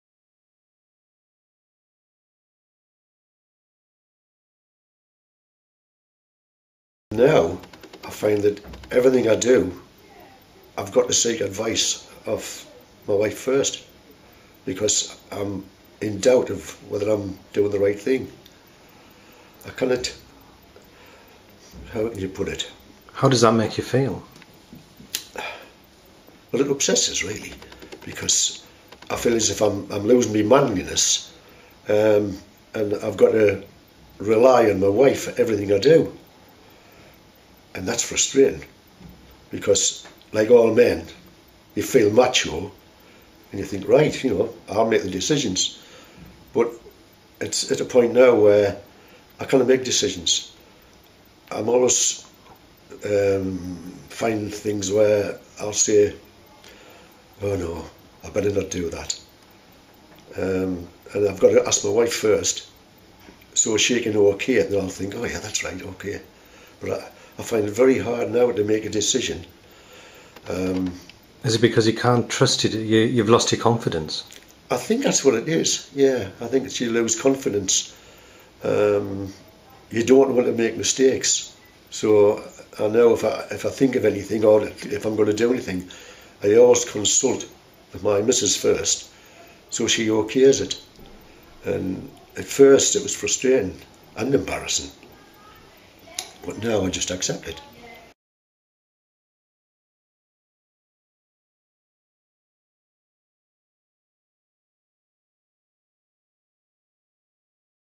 Living with Alzheimer's - "I'm losing my manliness."
Jack Fowler, speaking at St Cuthbert's Hospice, feels the disease is making him lose confidence and  is robbing him of his pride as a man.